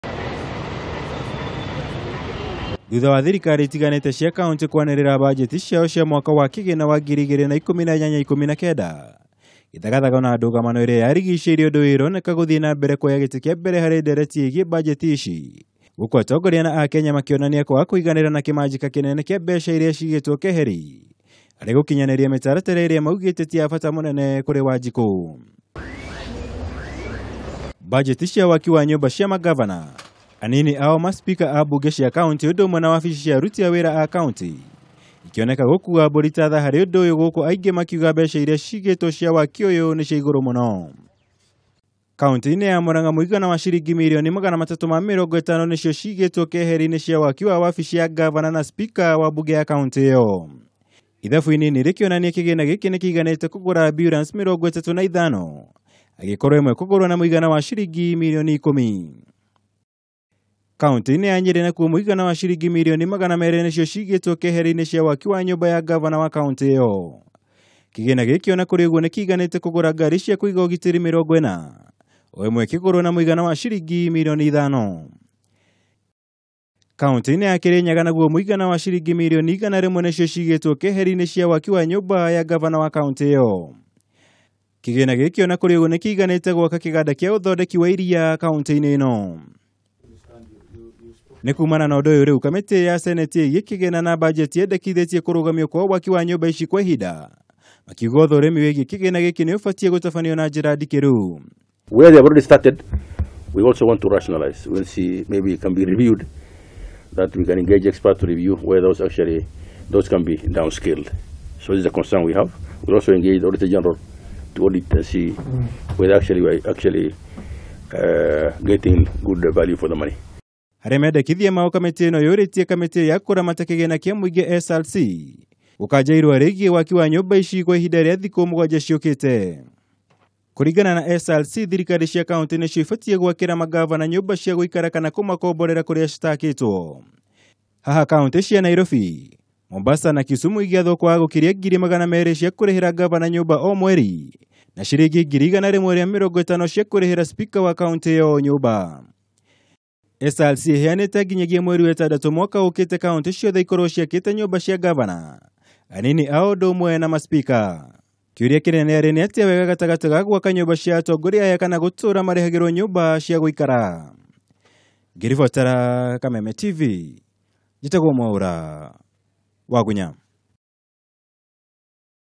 Nyumba cia atongoria a kaunti niciagiriirwo gwikirirwo mbere ya mathina ma Wanjiku?
Kamiti ya senate igii kigina na budget niyendekithitie kurugamio kwa waki wa nyumba cia mangavana na wabici ingi cia makaunty kwa ihinda nginya uthurimi wa kigina kiria kiigitwo keheri-ini kia waki wacio utabanio na urikiriire.
Ikirekereria mendekithia maya kamiti ino ithingatite kamiti ya kuramata micara ya muingi SRC gukanja watho wa waki wa nyumba ici budget-ini ya mwaka wa 2018/19 ihinda-ini ria thiku mugwanja, makiuga kigina kiria kiamuritwo hari waki wa nyumba ici ni kia iguru muno na nikirabatara kurorwo keri.